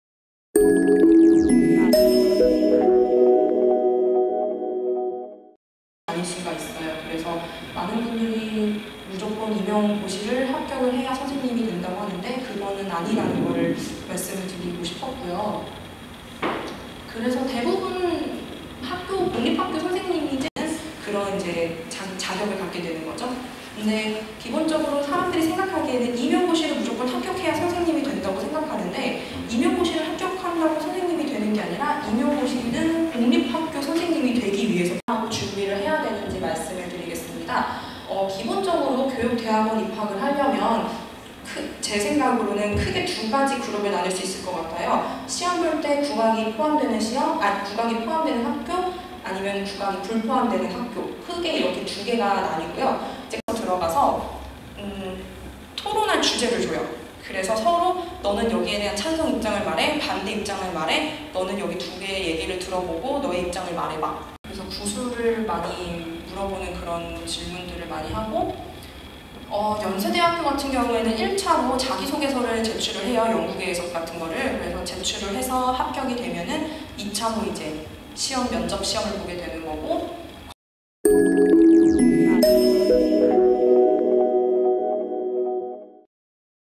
[싹튜브] 음악예술계열 졸업생 최숙은 음악 교육대학원 진로 특강 서울종합예술실용학교
음악예술계열 졸업생 최숙은 음악 교육대학원 진로 특강